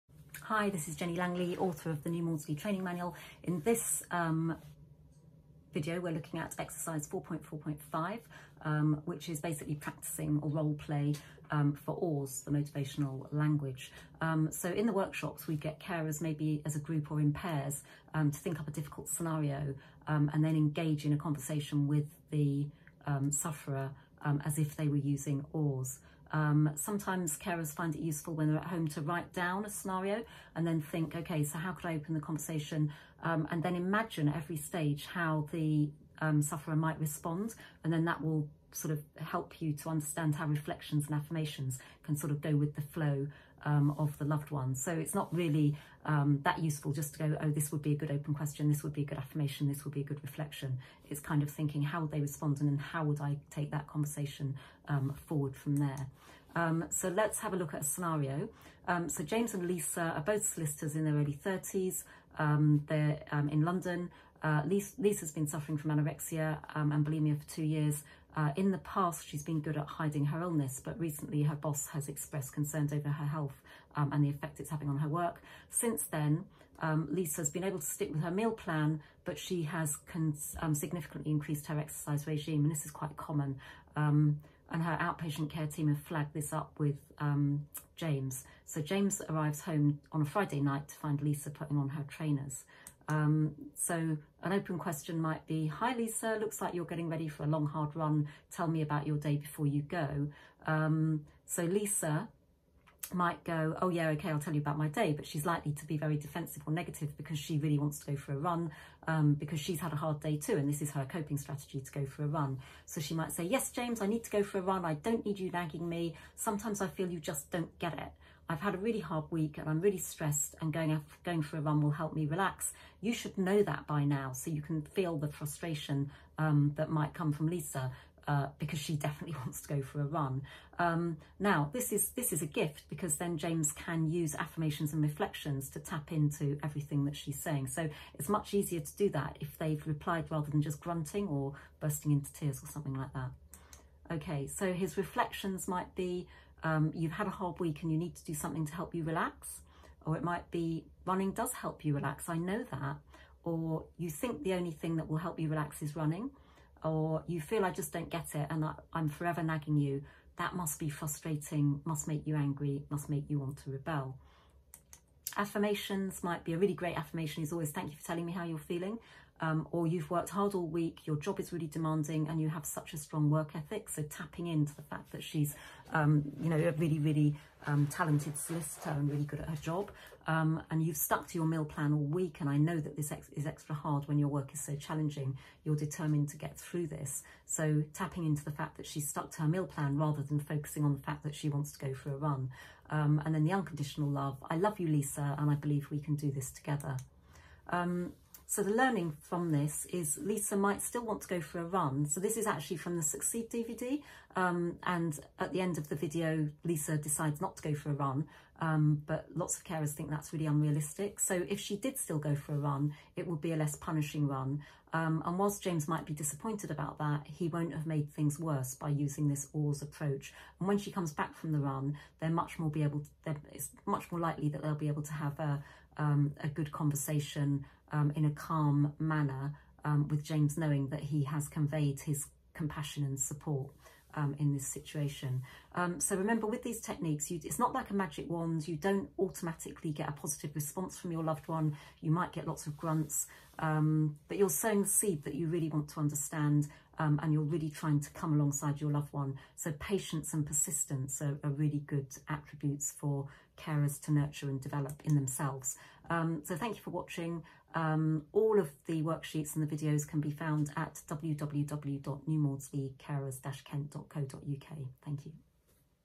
New Maudsley Training Manual Exercise 4.4.5 OARS Role Play
This video is about New Maudsley Training Manual Exercise 4.4.5 OARS Role Play. The best way to practice OARS communication skills is to think up a difficult scenario and imagine you are having a conversation with your loved one using the OARS approach.